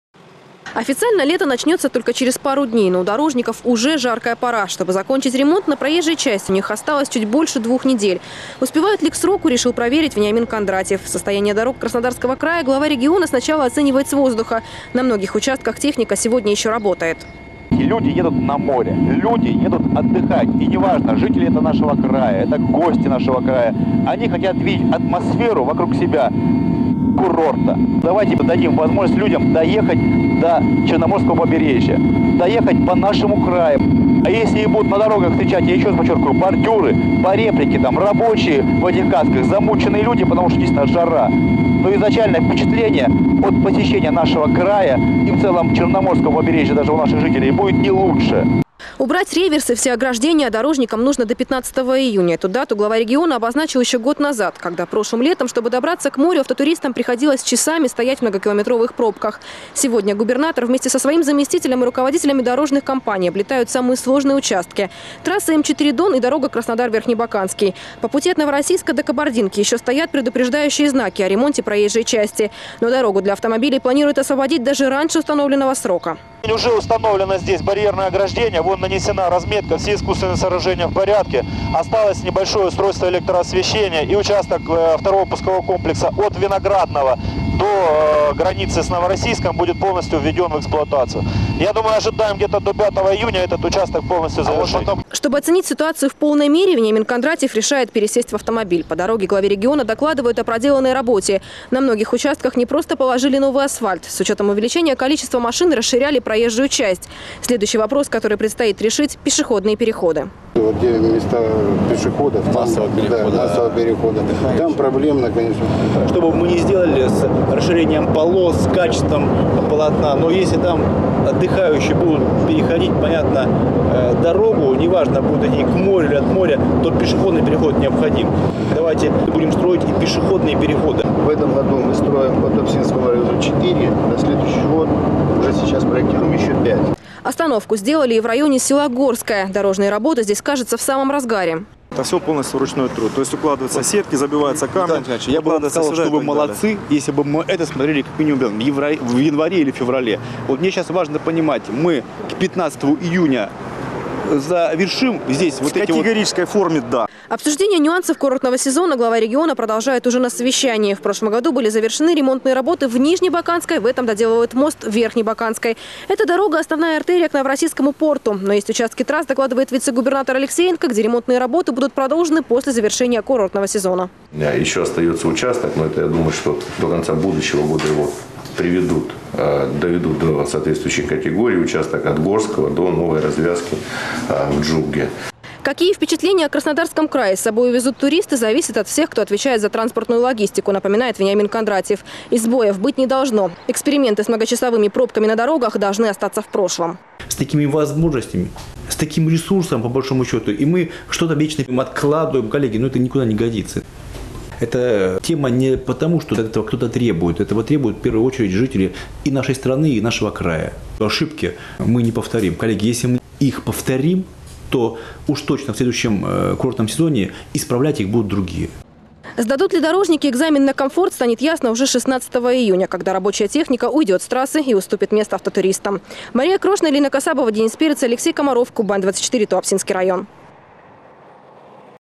Убрать реверс и все ограждения дорожникам нужно до 15 июня. Новости Сочи Эфкате
Две недели на завершение всех работ. Столько времени осталось у дорожников, чтобы убрать с проезжей части всю технику. Успевают ли к сроку закончить ремонт автомагистралей Кубани сегодня проверил Вениамин Кондратьев.
Сочи. Новости.
Телеканал Эфкате.